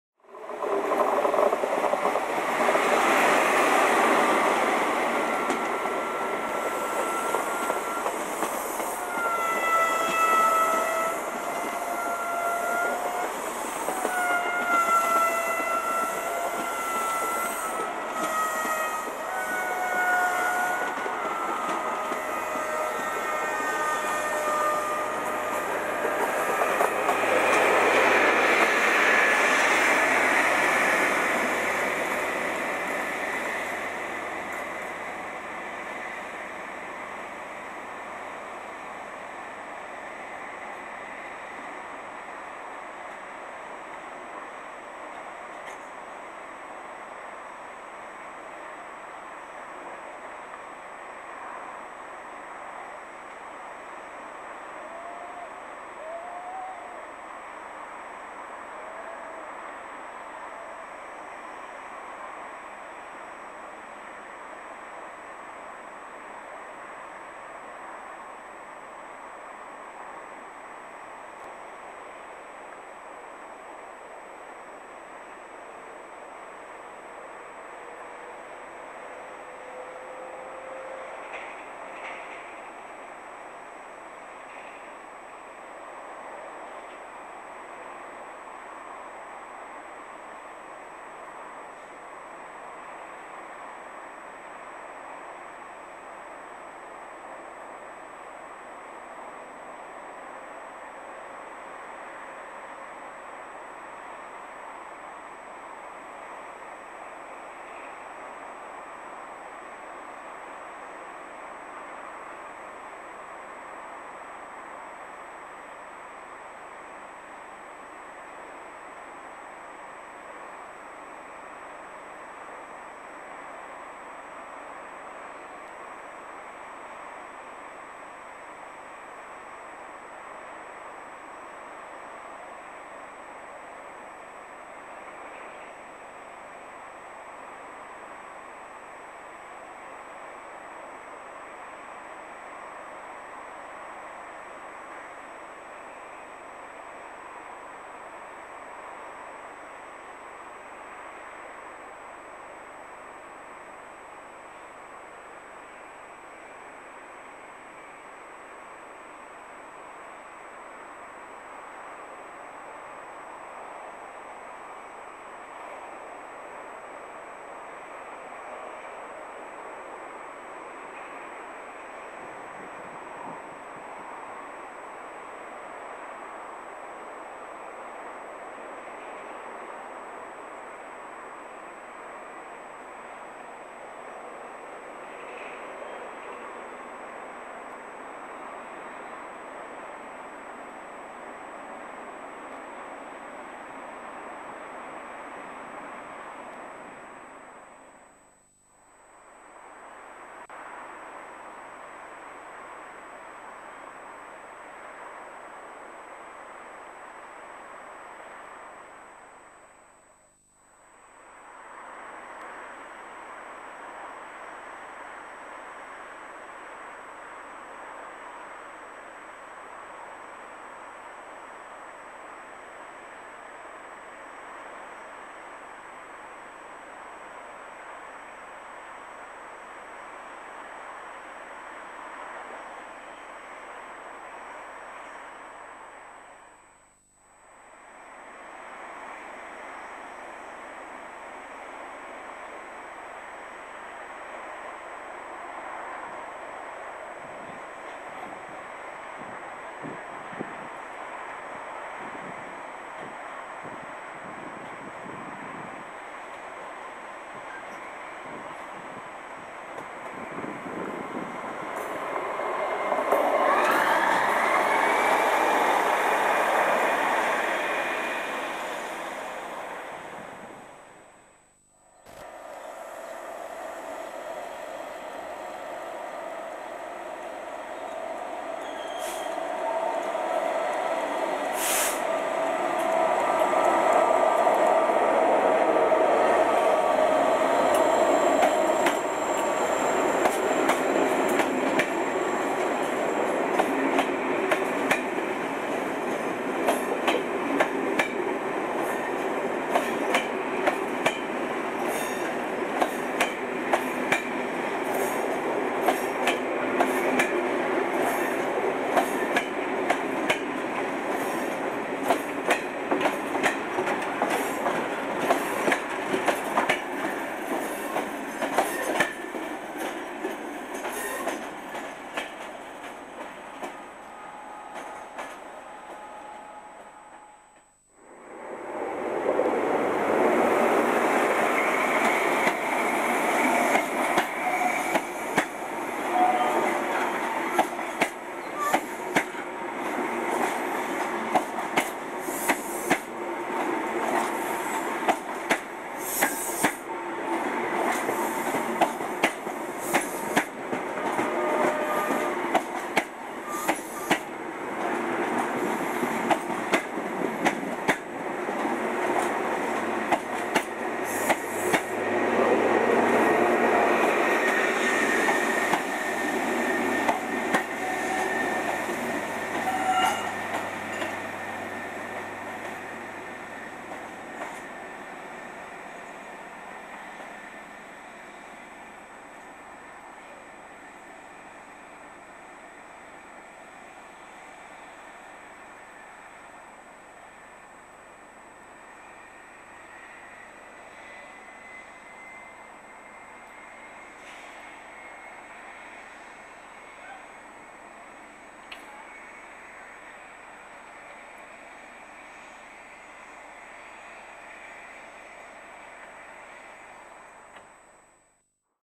Tamar Bridge 2002
See various services crossing into or from Cornwall over the Tamar Bridge. Includes DMU, HST and class 67